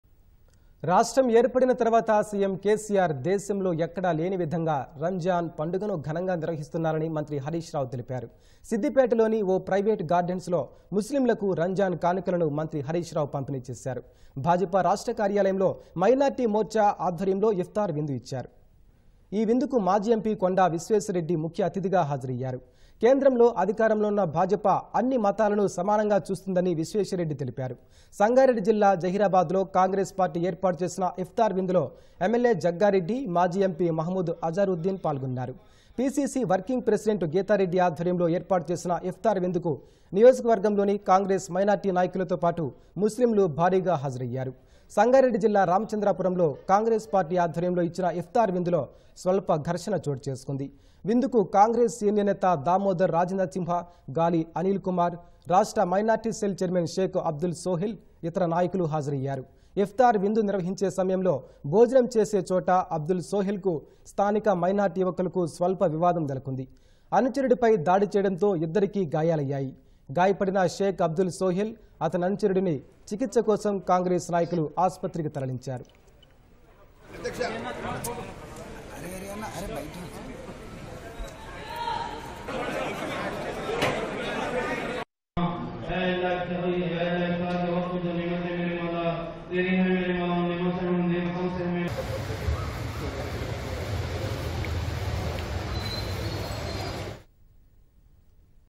Iftar Feast | Minister Harish Rao Participated | Siddipet Dist
రాష్ట్రం ఏర్పడిన తర్వాత సీఎం కేసీఆర్  దేశంలో ఎక్కడా లేని విధంగా... రంజాన్  పండుగను ఘనంగా నిర్వహిస్తున్నారని మంత్రి హరీశ్ రావు తెలిపారు. సిద్దిపేటలోని ఓ ప్రైవేటు గార్డెన్స్ లో ముస్లింలకు రంజాన్ కానుకలను... మంత్రి హరీశ్ రావు పంపిణీ  చేశారు. భాజపా రాష్ర్ట కార్యాలయంలో మైనార్టీ మోర్చా ఆధ్వర్యంలో ఇఫ్తార్  విందు ఇచ్చారు. ఈ విందుకు మాజీ ఎంపీ కొండా విశ్వేశ్వర్ రెడ్డి ముఖ్య అతిథిగా హాజరయ్యారు. కేంద్రంలో అధికారంలో ఉన్న భాజపా అన్ని మతాలను సమానంగా చూస్తుందని విశ్వేశ్వర్ రెడ్డి  తెలిపారు. సంగారెడ్డి జిల్లా జహీరాబాద్ లో కాంగ్రెస్ పార్టీ ఏర్పాటు చేసిన ఇఫ్తార్ విందులో.. ఎమ్మెల్యే జగ్గారెడ్డి, మాజీ MP మహమ్మద్ అజహరుద్దీన్  పాల్గొన్నారు. పీసీసీ వర్కింగ్ ప్రెసిడెంట్ గీతారెడ్డి ఆధ్వర్యంలో  ఏర్పాటు చేసిన ఇఫ్తార్ విందుకు... నియోజకవర్గంలోని కాంగ్రెస్ మైనార్టీ నాయకులతో పాటు ముస్లింలు భారీగా హాజరయ్యారు. సంగారెడ్డి జిల్లా రామచంద్రాపురంలో కాంగ్రెస్ పార్టీ ఆధ్వర్యంలో ఇచ్చిన ఇఫ్తార్  విందులో... స్వల్ప ఘర్షణ చోటు  చేసుకుంది. విందుకు కాంగ్రెస్  సీనియర్  నేత దామోదర్ రాజనర్సింహ, గాలి అనిల్ కుమార్, రాష్ట్ర మైనార్టీ సెల్ ఛైర్మన్ షేక్ అబ్దుల్ సోహెల్ ఇతర నాయకులు హాజరయ్యారు.  ఇఫ్తార్  విందు నిర్వహించే సమయంలో భోజనం చేసేచోట.. అబ్దుల్ సోహెల్ కు స్థానిక మైనార్టీ యువకులకు స్వల్ప వివాదం నెలకొంది. అనుచరుడిపై దాడి చేయడంతో ఇద్దరికీ గాయాలయ్యాయి. గాయపడిన షేక్ అబ్దుల్ సోహెల్, అతని అనుచరుడిని చికిత్స కోసం కాంగ్రెస్ నాయకులు ఆస్పత్రికి తరలించారు.